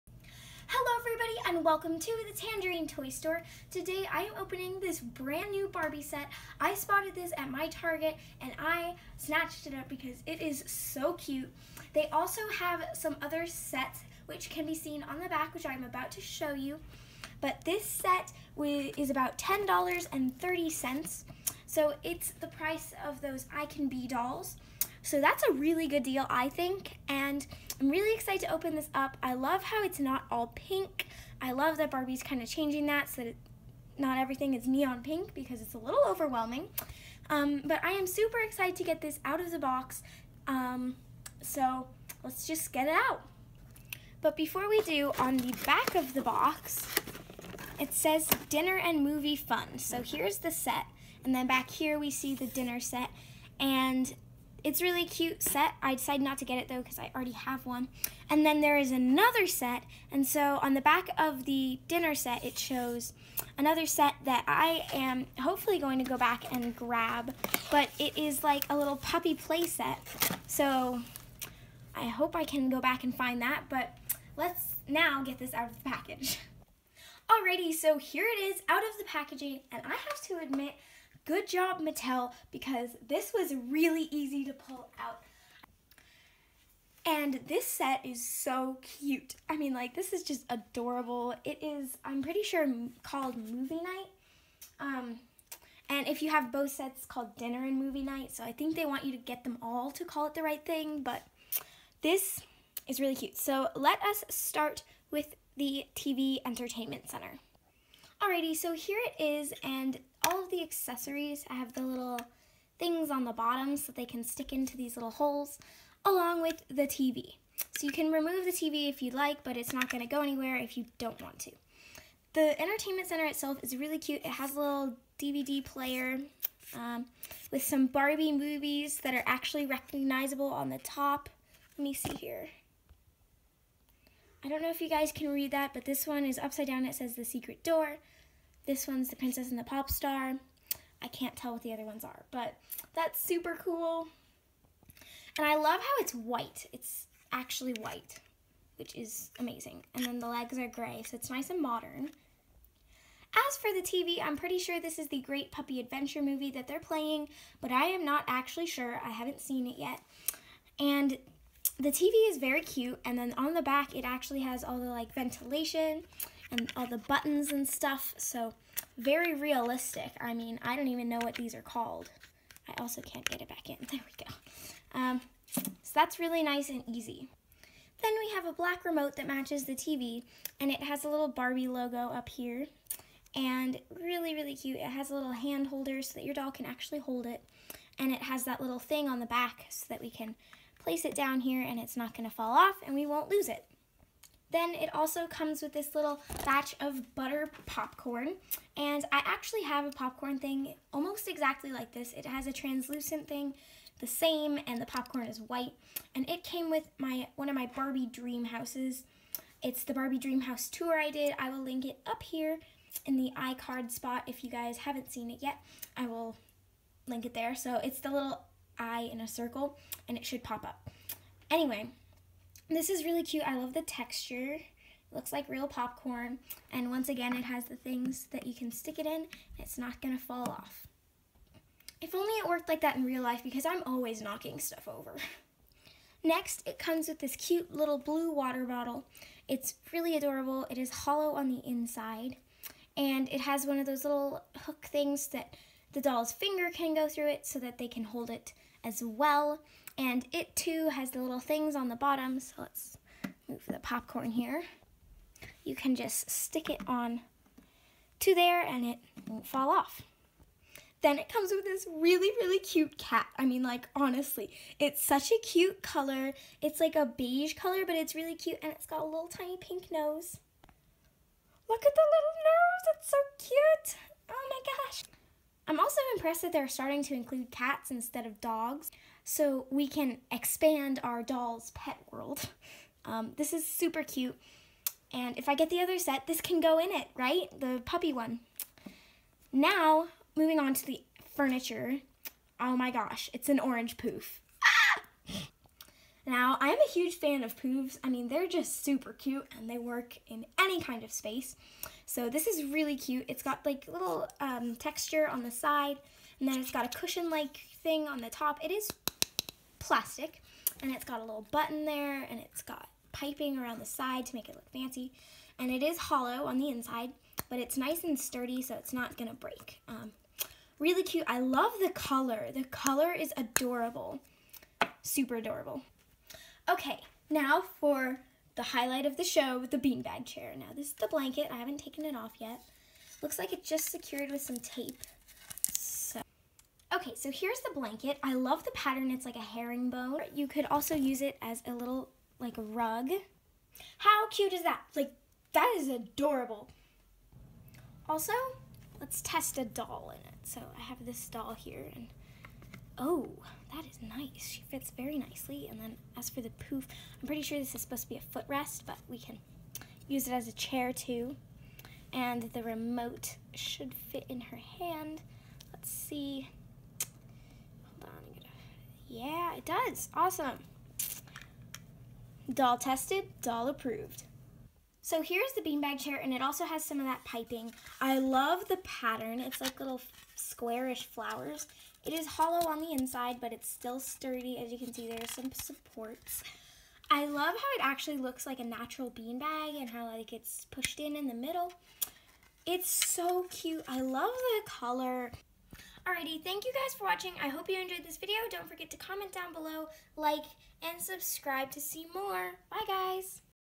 NEW! Barbie Movie Set Review!
This is an adorable set for an amazing price and for one it is not all pink!
Thank you all so much for watching! I hope you enjoyed this video, if you liked it please give this vid a big thumbs up! 🍊🍊